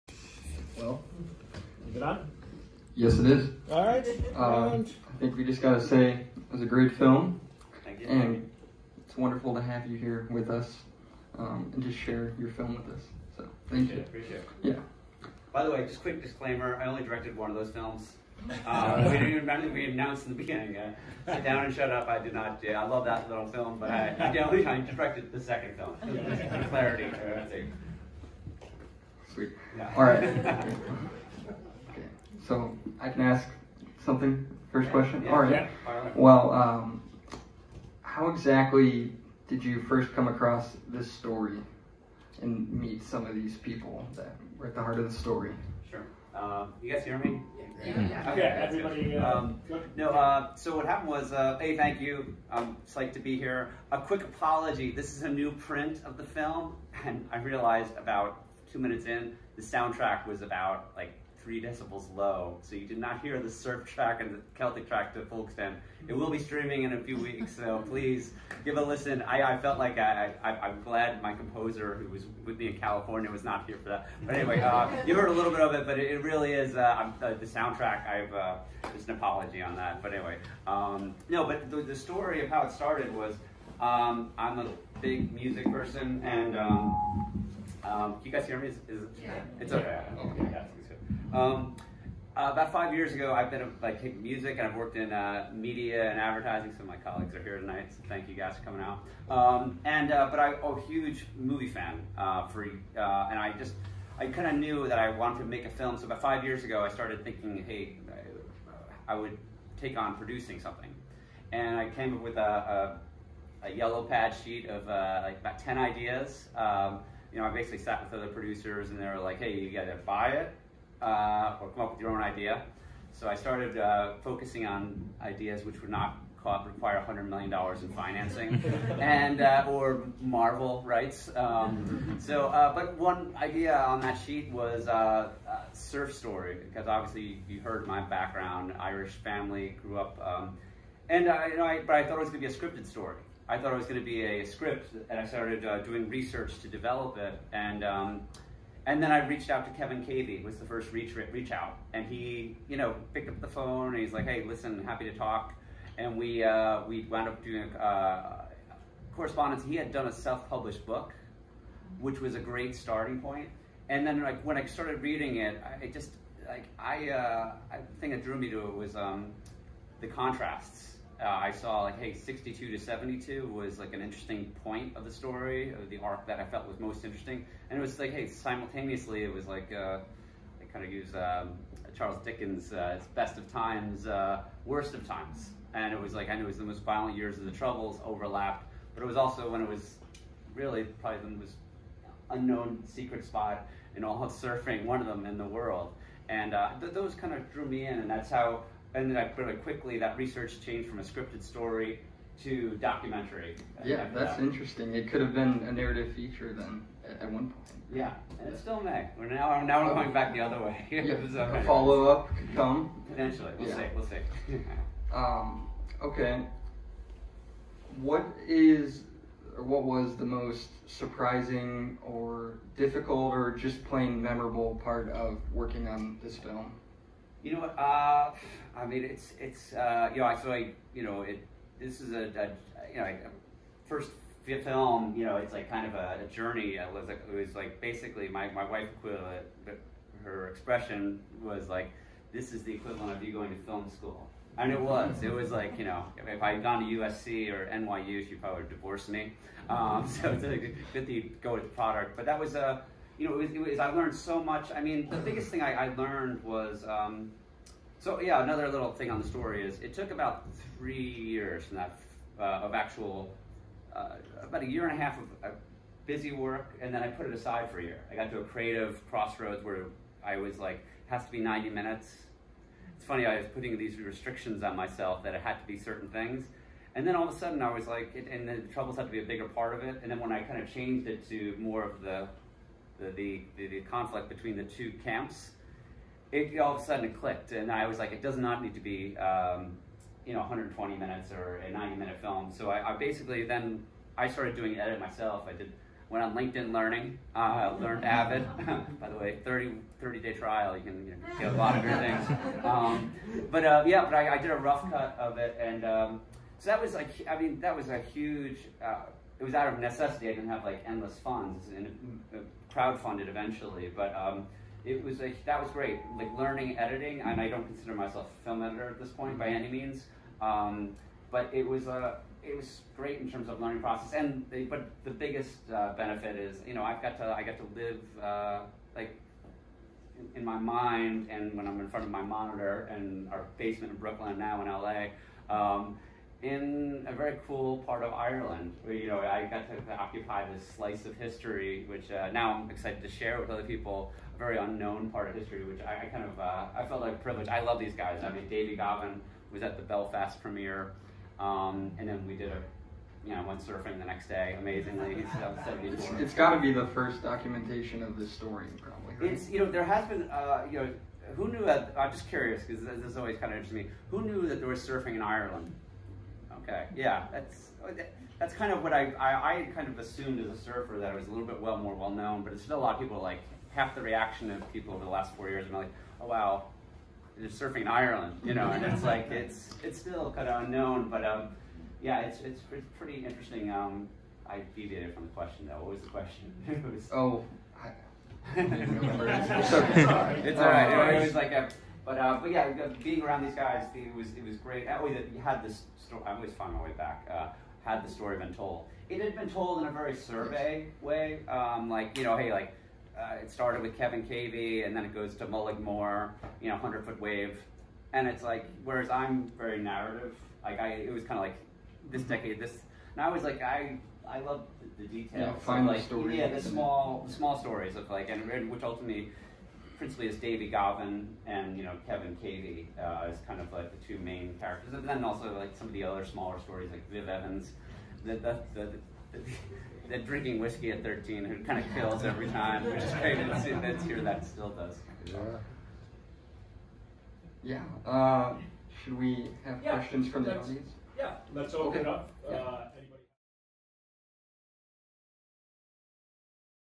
Keep It A Secret -- My Q&A with Documentarian Sean Duggan
"Keep It A Secret" is the inspiring true story of the dawn of Irish surfing and how the sport's brave pioneers found peace and escape in the water during the most violent years of The Troubles in Northern Ireland.